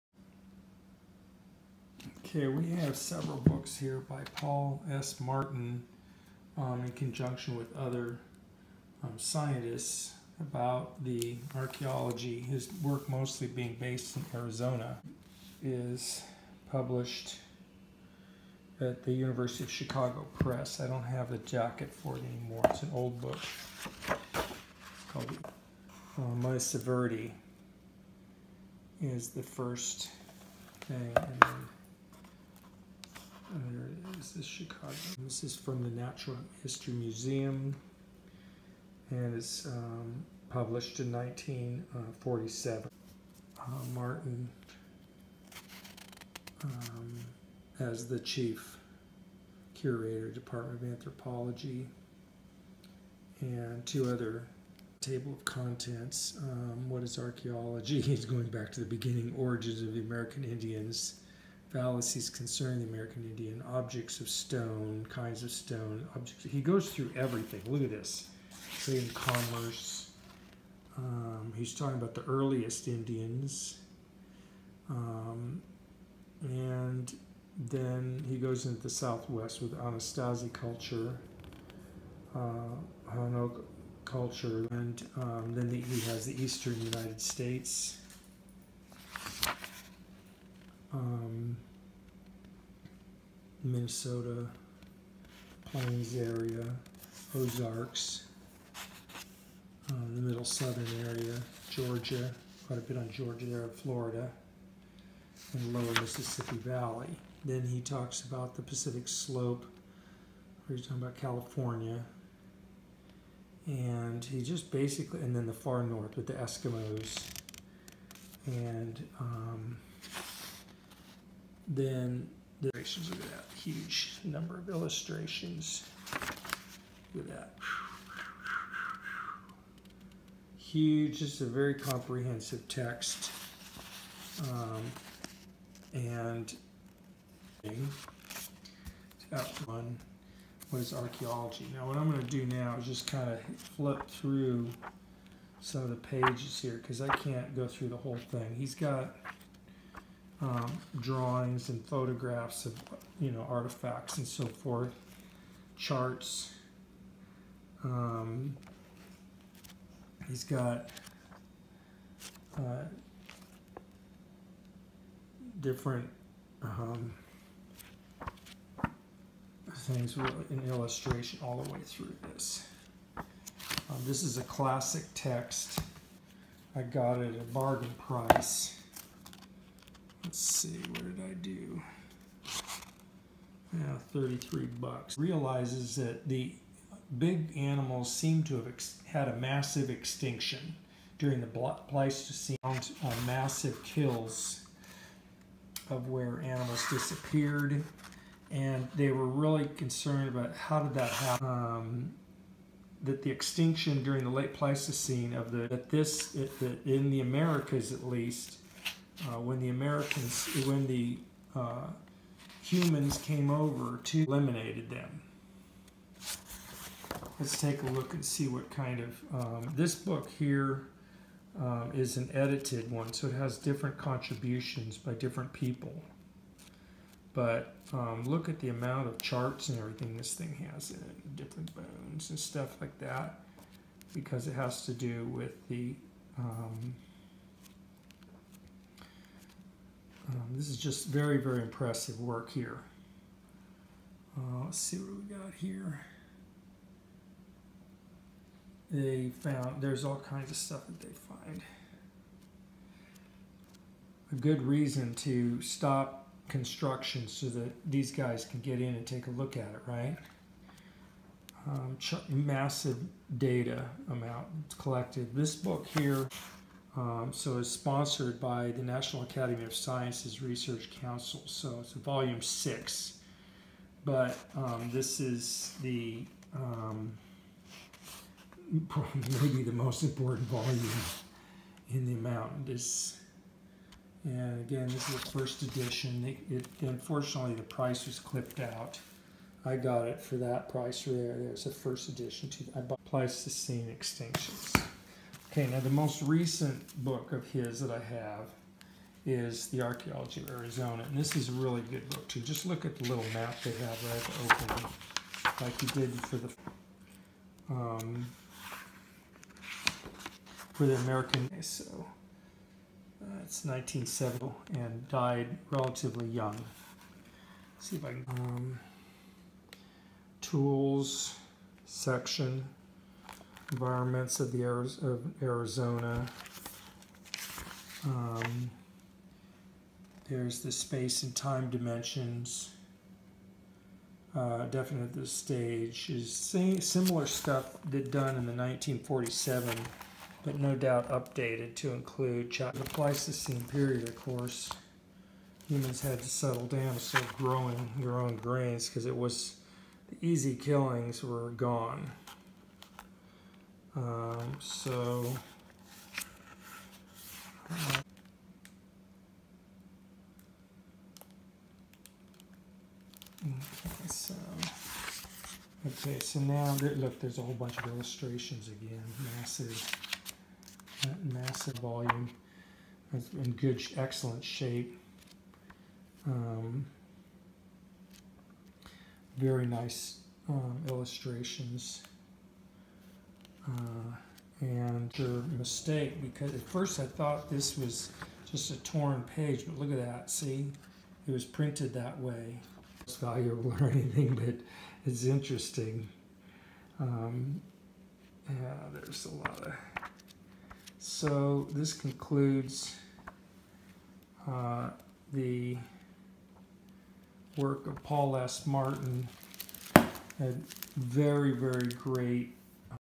Legendary Archeologist Paul S Martin (1947 to 1973)
Paul S Martin was a major scientist of 20th Century for his Theory of Pleistocene Extinctions